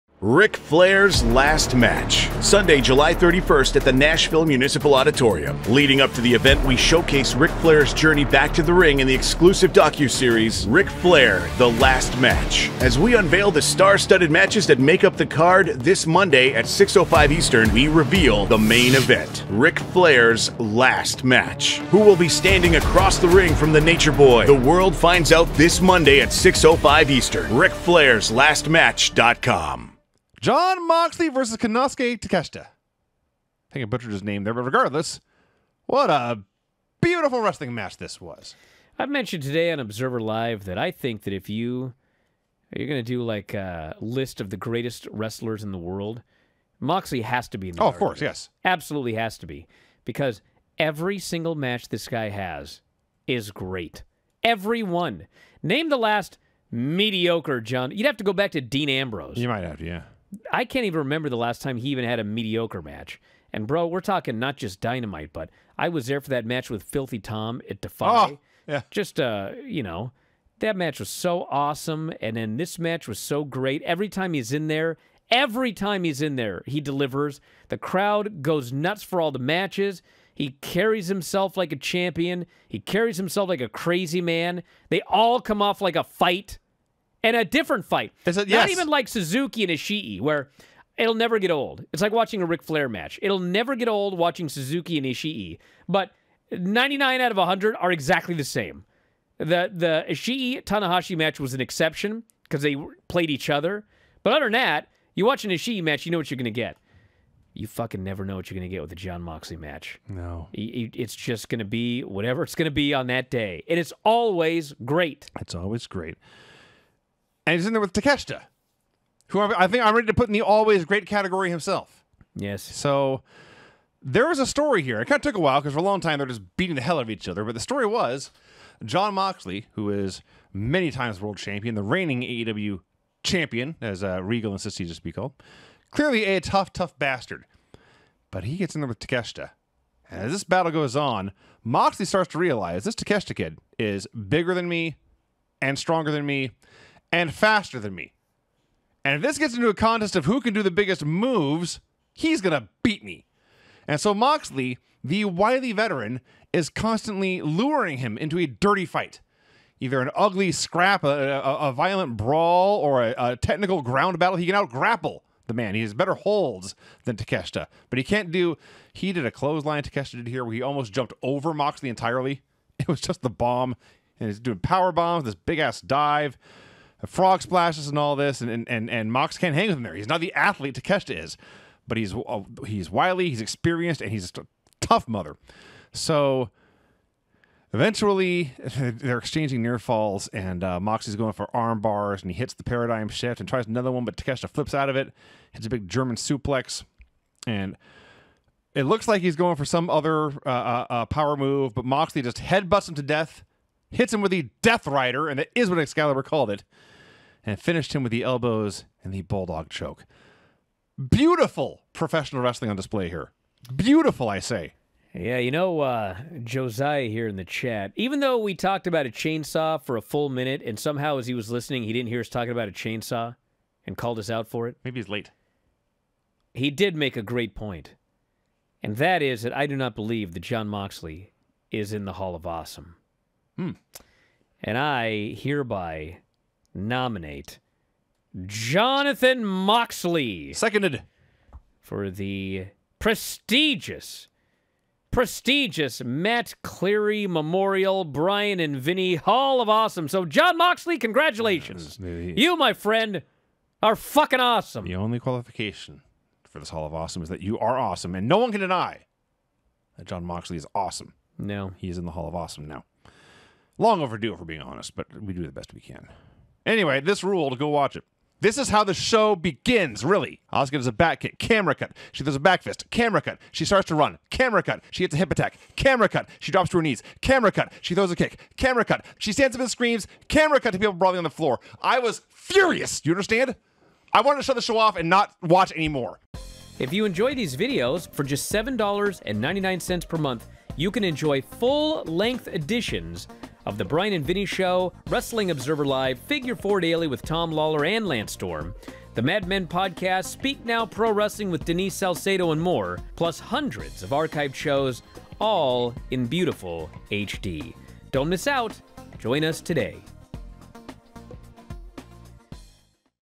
Moxley vs. Takeshita was awesome pro wrestling: Bryan & Vinny Show
Bryan and Vinny share their thoughts on the Jon Moxley vs. Konosuke Takeshita match that took place on this week's Dynamite. [July 14, 2022]